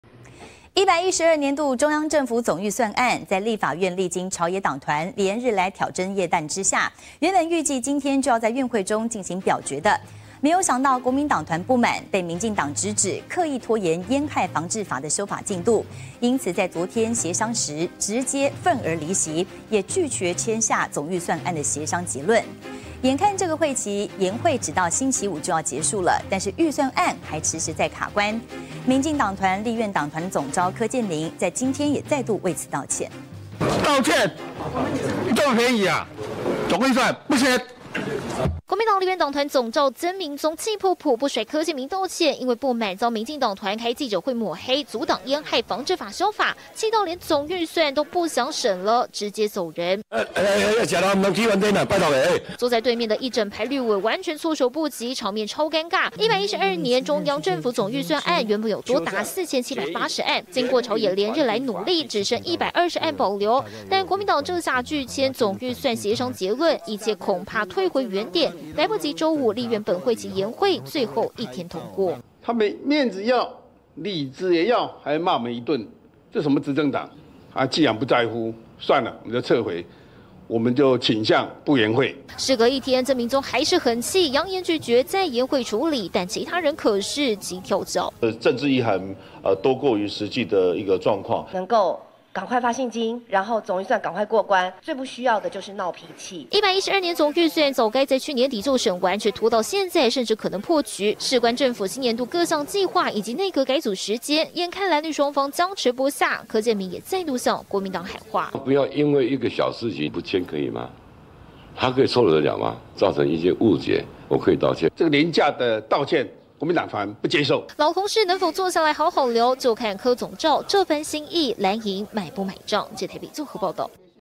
不滿被抹黑 藍拒簽總預算協商結論
112年度中央政府總預算案，在立法院歷經朝野黨團，連日來挑燈夜戰，原本預計今天(12號)將在院會中進行表決，不料國民黨團不滿，遭民進黨直指刻意拖延菸害防制法修法進度，昨天(11號)在協商時直接憤而離席，也拒簽總預算案協商結論。眼看本會期延會只到週五就要結束，預算案卻遲遲卡關，民進黨立院黨團總召柯建銘，今天(12號)也為此再度道歉。